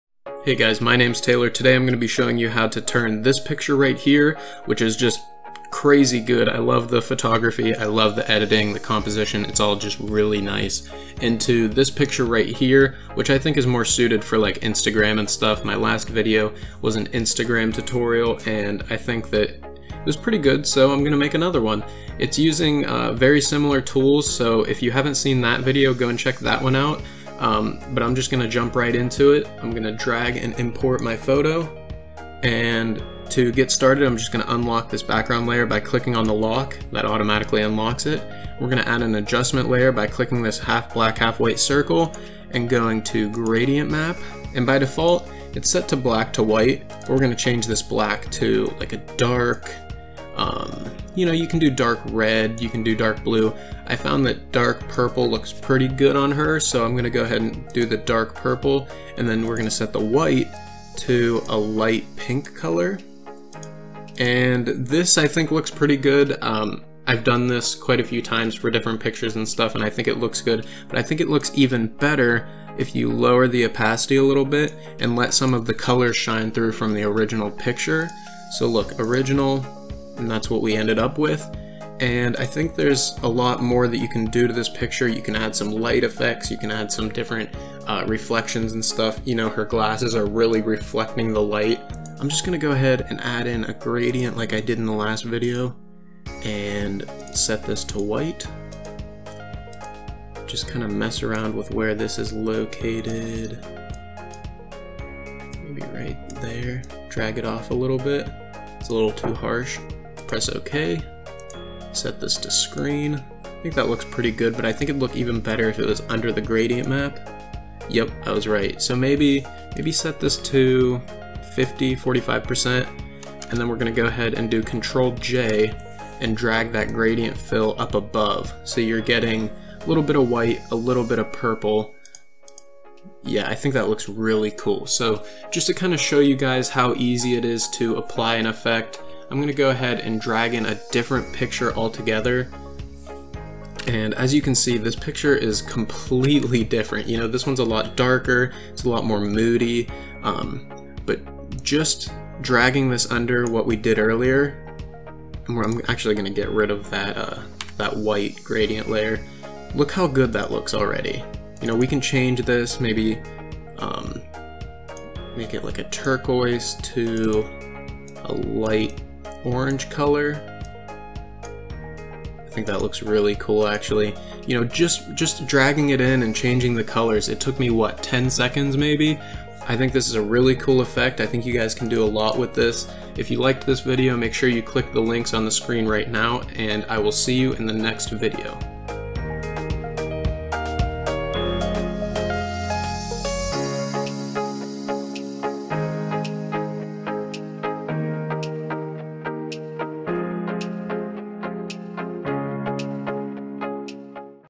Instagram Creative Editing - Photoshop Tutorial
How to creatively edit photos for your Instagram! This works best for photos that have already been color corrected but color correcting isn't necessary! A beginner can do this easily, just follow along through my tutorial. All you need for this are pictures to edit, Adobe Photoshop (Preferably CC but I'm sure anything past CS3 would work) and an Instagram profile so you can upload your photos and get famous! ;) If you have any ideas for future tutorials make sure you post a comment and I will try to do that. I work in Photoshop and Premiere Pro but I'm more than willing to branch outside of those and make some how to tutorials for everyone.

The song I used in the background is a copyright-free song which you can find right here:

Show them some love and make sure to show me some love by liking and commenting!